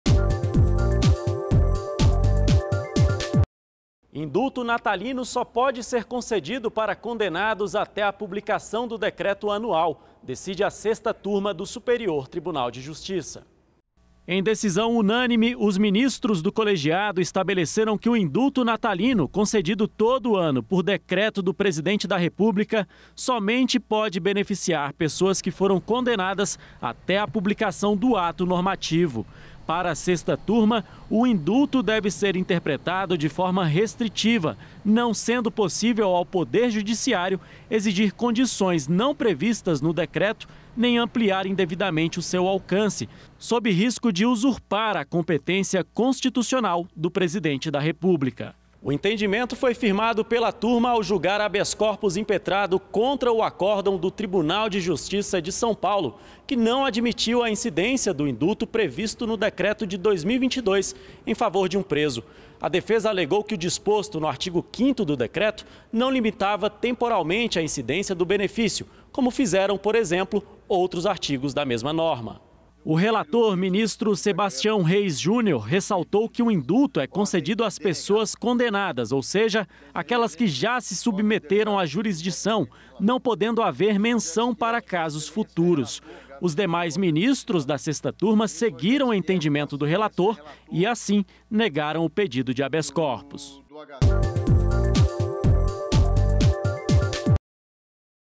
Indulto natalino só pode ser concedido a quem foi condenado até a publicação do decreto
​A Sexta Turma do Superior Tribunal de Justiça (STJ), em decisão unânime, estabeleceu que o indulto natalino, concedido todo ano por decreto do presidente da República, somente pode beneficiar pessoas que foram condenadas até a publicação do ato normativo.